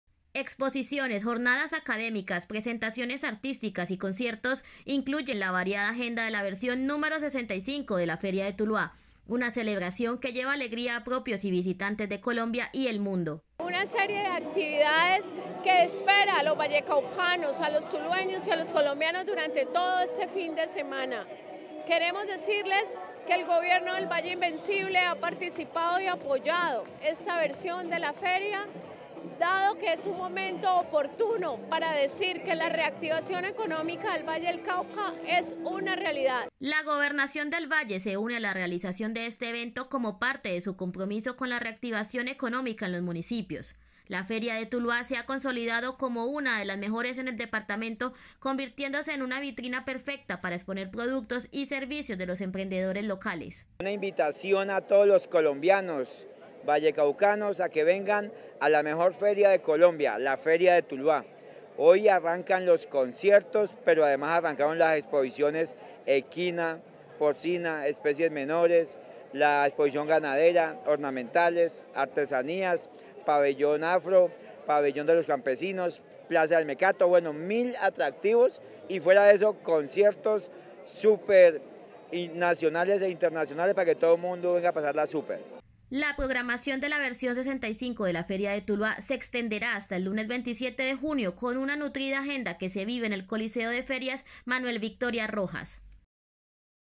Feria de Tuluá, una vitrina para la economía y la diversión
Emitido el 25  de junio 2022 Dia   por el Canal regional Telepacífico.

La Gobernadora encargada del Valle, María Leonor Cabal, acompañó la inauguración de la versión 65 de la Feria de Tuluá, que ofrece una variada programación con exposiciones académicas, artesanías, plaza del mecato, presentaciones artísticas y conciertos.